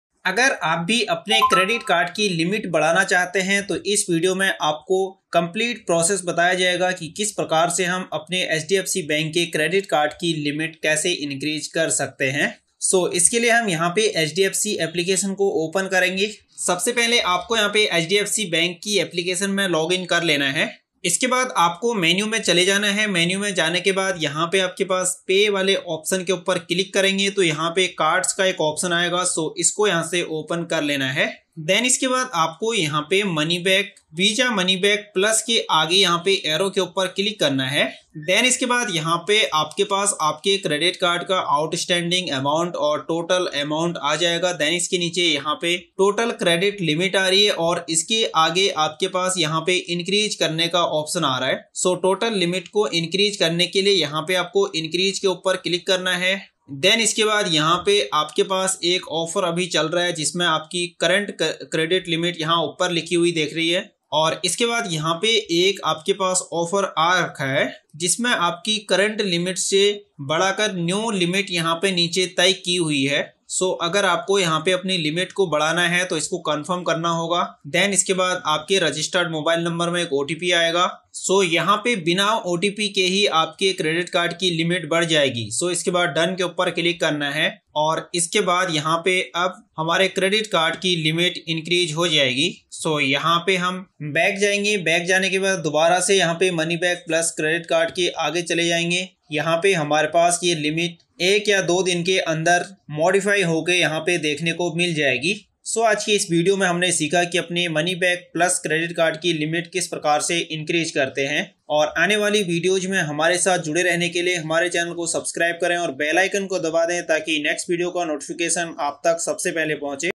how to increase hdfc bank credit card limit? credit card limit kaise badhaye?hdfc bank credit cards?
In this video we will learn how to increase the hdfc bank credit card limit in Hindi?

Personal Watch
Personal Laptop
Personal Camera
Personal Wireless Keyboard & Mouse
Personal Mic